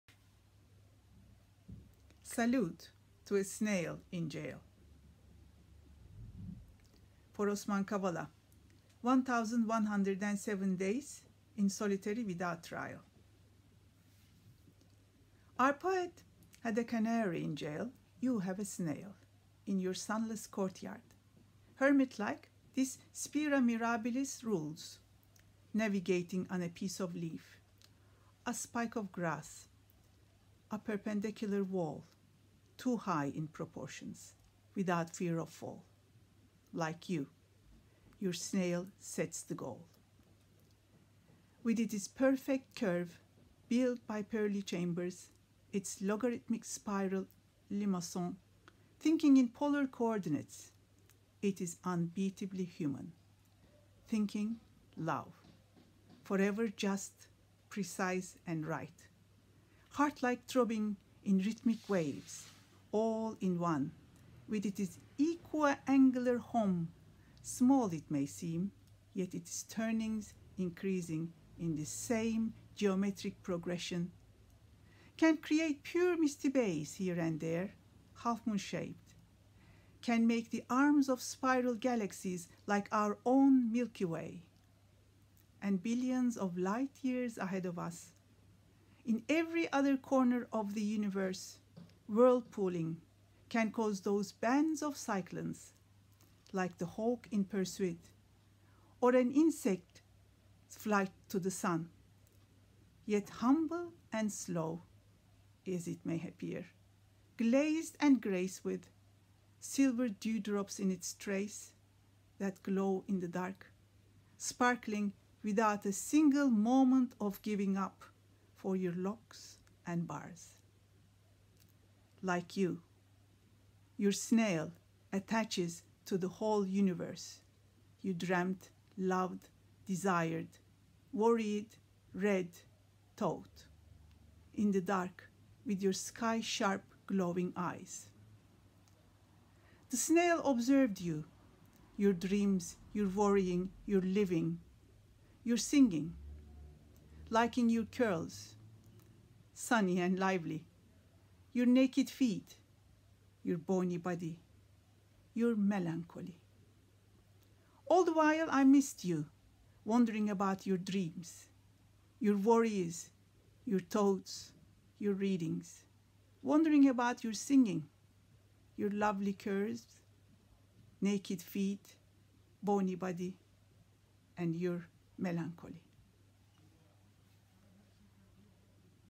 Salute to a Snail by Sebnem Senyener
This poem appears in the Beltway Poetry Quarterly "Art in Times of Crises, Volume II".  Sebnem Senyener has published six novels. Born in Turkey she lives now in London.